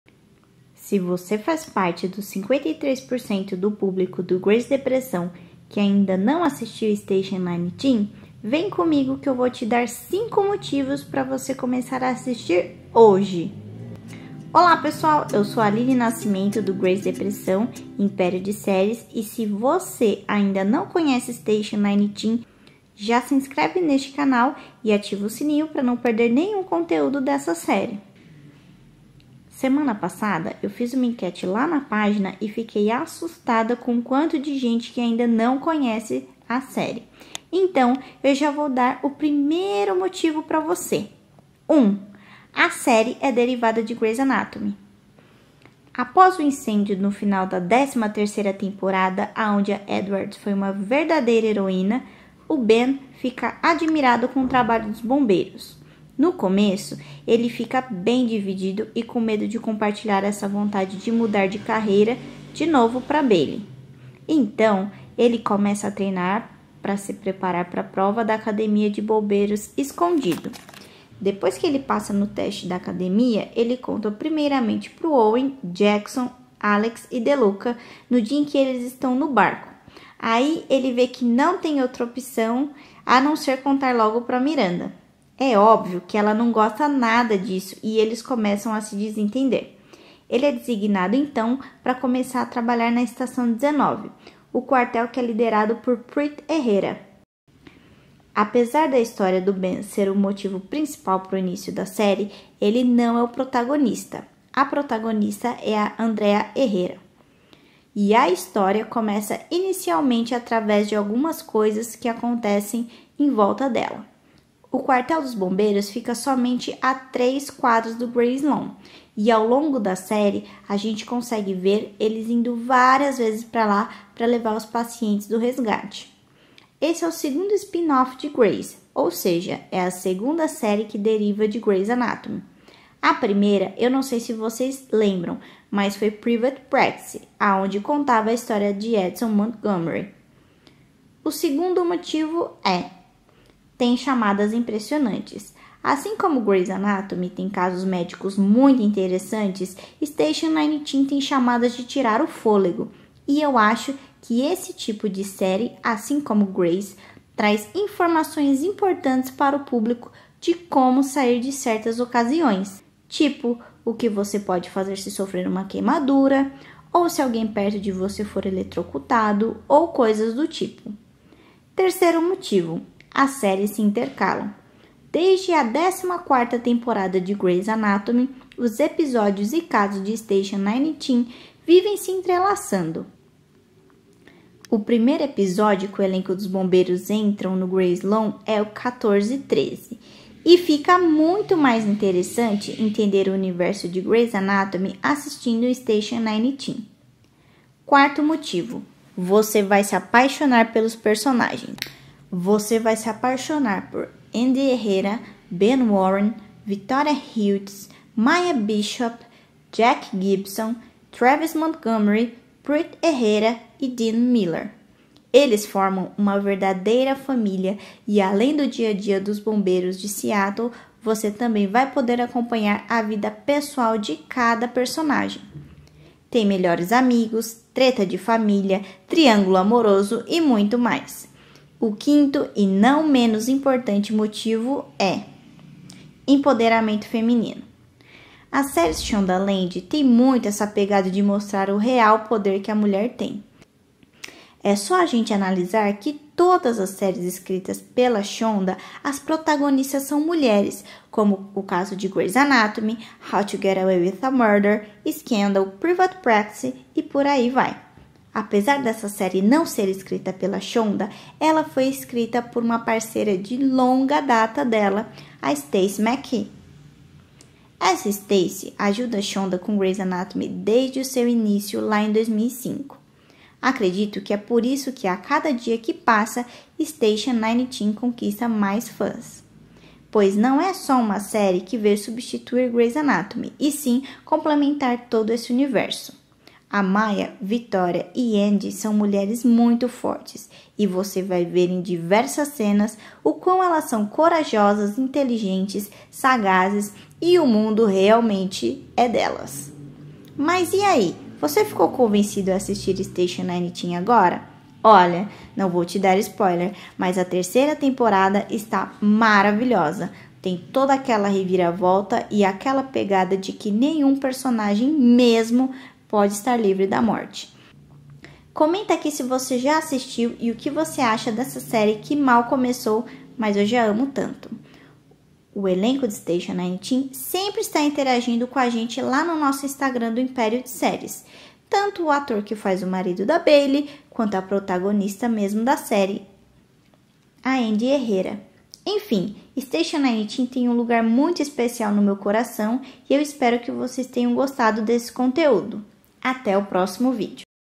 5 MOTIVOS PARA VOCÊ ASSISTIR STATION 19 HOJE!
Você já conhece a série Station 19?
Hoje eu vou dar pra vocês 5 motivos para começar a assistir Station 19.

Siga meu Instagram para ficar por dentro das pautas para o próximo vídeo:

Agora estamos no Telegram, entre no nosso grupo para receber atualizações de Grey's Anatomy: